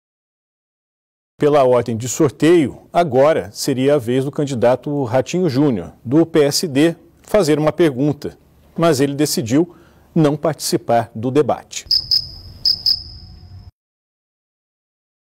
O Rato fugiu 🐀
Ratinho Júnior não comparece ao debate!